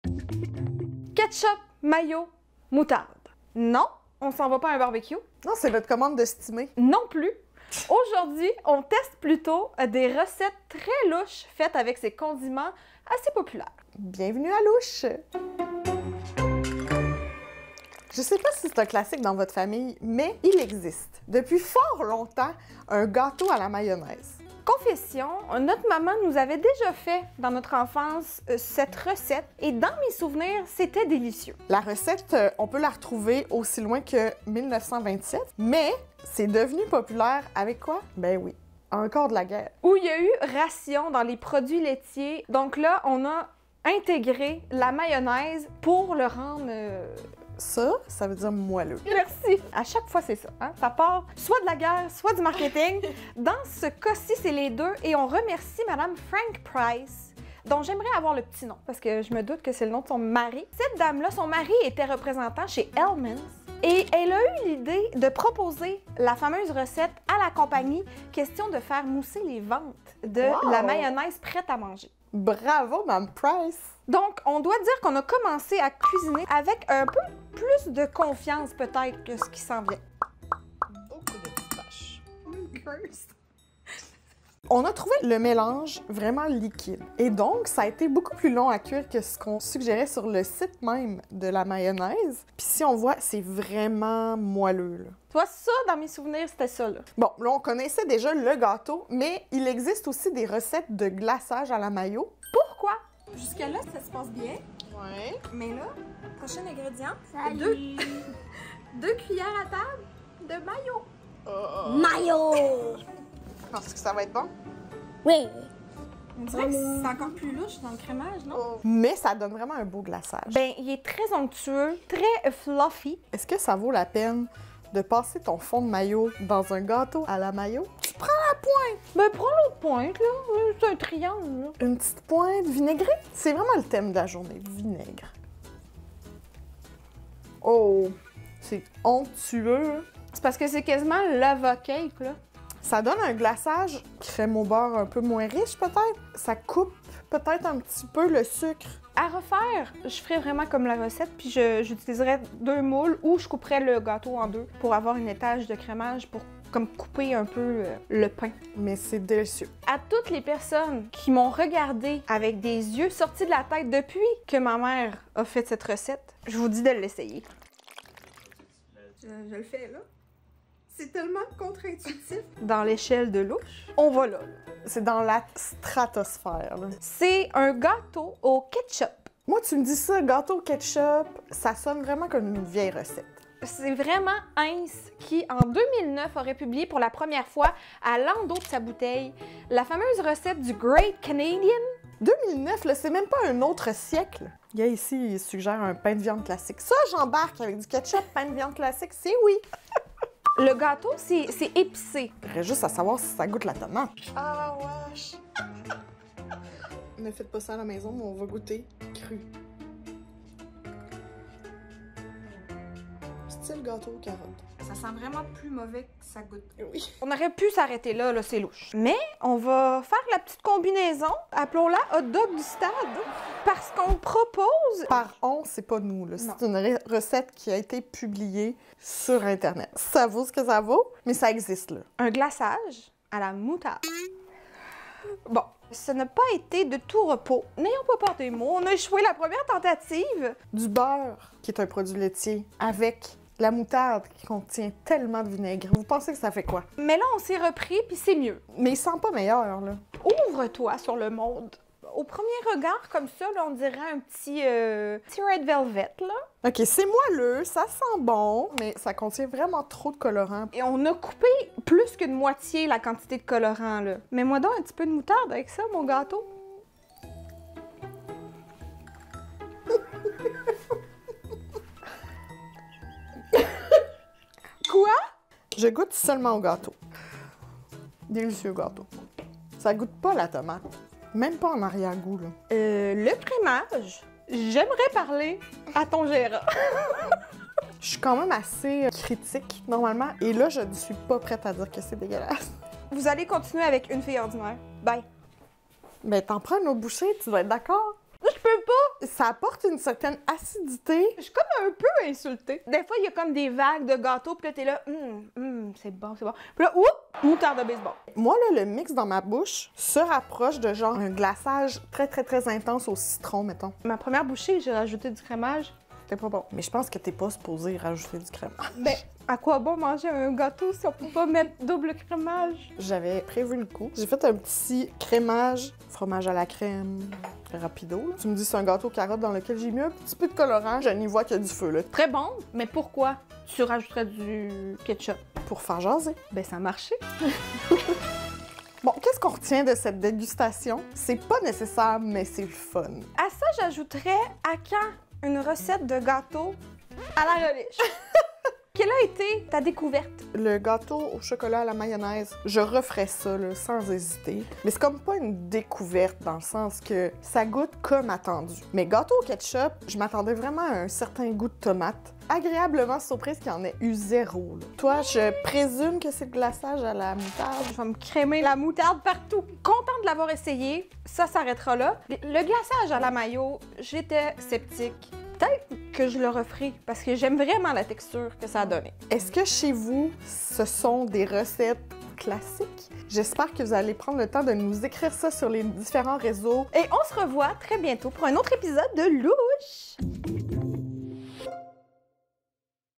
2FillesOrdinaires testent des gâteaux au ketchup, à la mayo et à la moutarde!
Ne reculant devant rien, Chrystine et Vanessa essaient des desserts à la mayonnaise et au ketchup, elles font aussi un glaçage à la moutarde. Les résultats sont surprenants et... très louches!